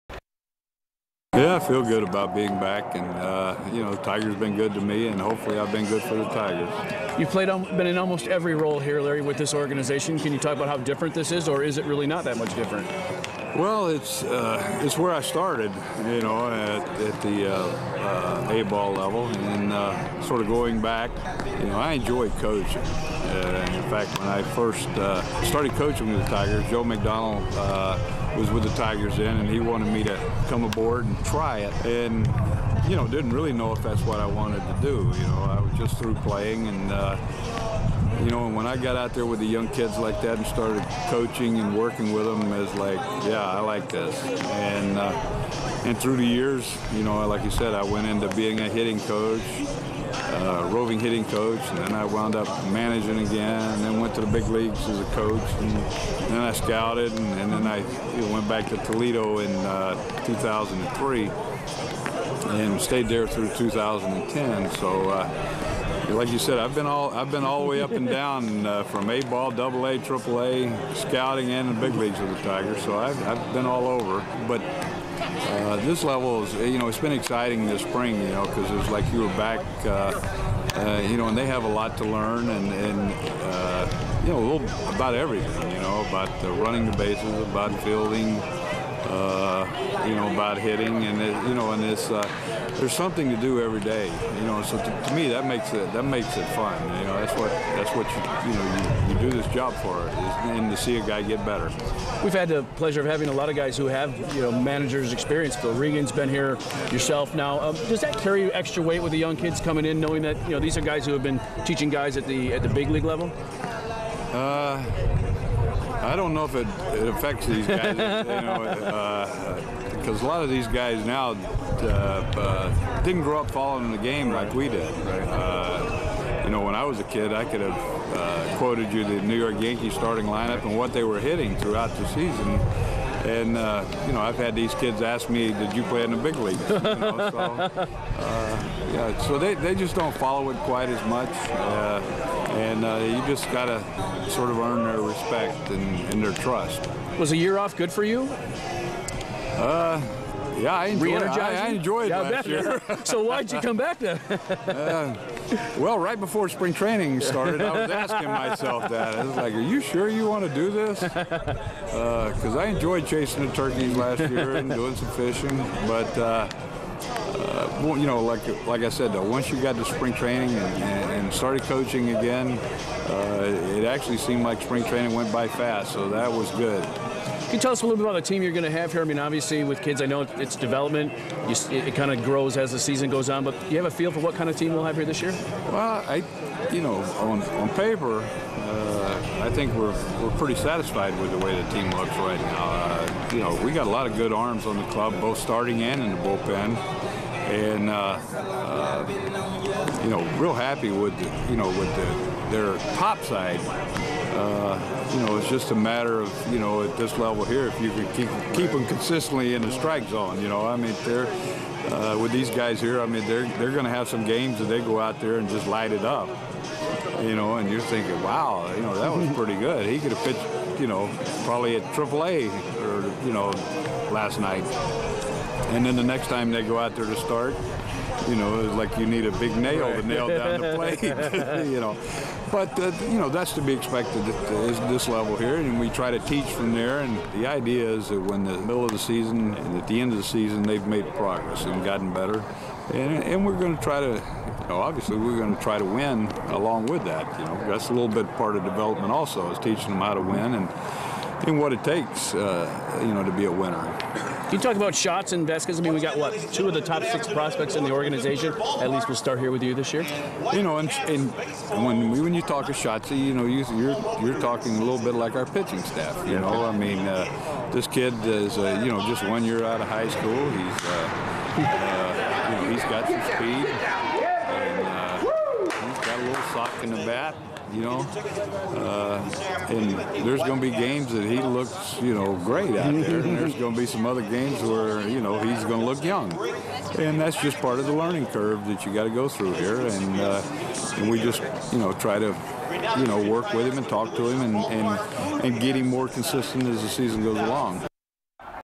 Sports OT: Whitecaps Manager Parrish
Larry Parrish spent years with Tigers organization -- now he's back.